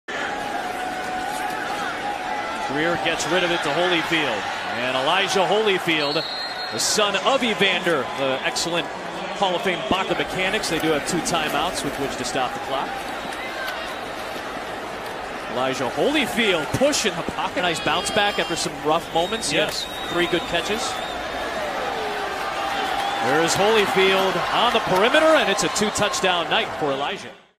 Elijah Holyfield Week 1 2019 Preseason Highlights
Elijah Holyfield ran for 26 yards on 9 rushes. He also caught 2 passes for 13 yards. He also had a rushing and receiving touchdown. Check out his highlights in his debut.